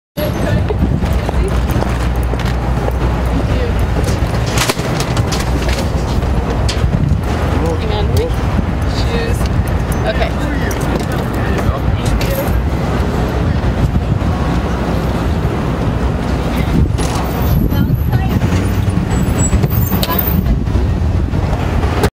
Elizabeth Berkley and Valentin Chmerkovskiy at the McGraw...
Elizabeth Berkley and Valentin Chmerkovskiy at the McGraw-Hill Building at Celebrity Sightings in New York in New York, NY, on 11/13/2013.